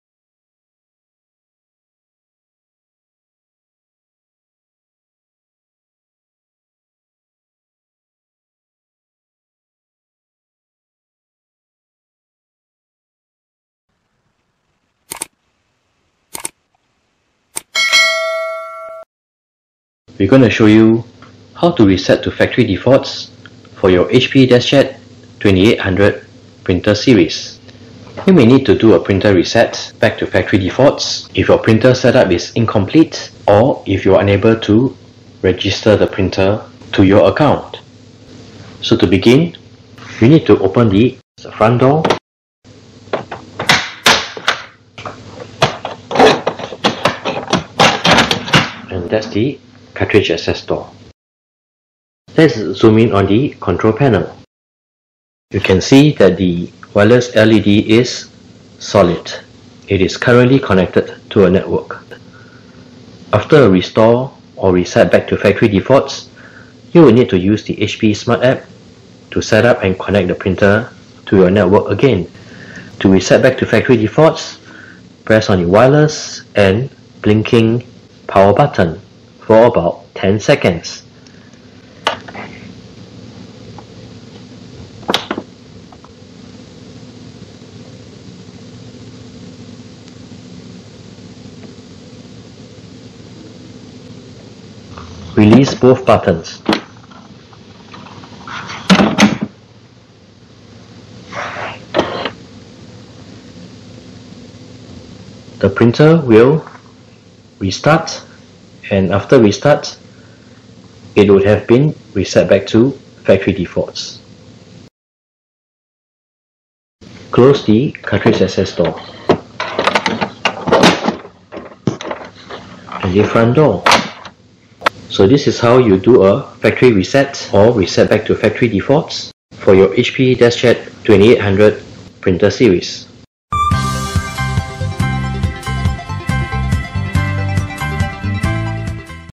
HP DeskJet 2855e | 2810e | 2820e printer series : How to reset restore to factory defaults
This tutorial shows how to Reset | Restore to factory defaults for HP DeskJet 2800 Printer Series.

This printer supports Print, Scan & Copy.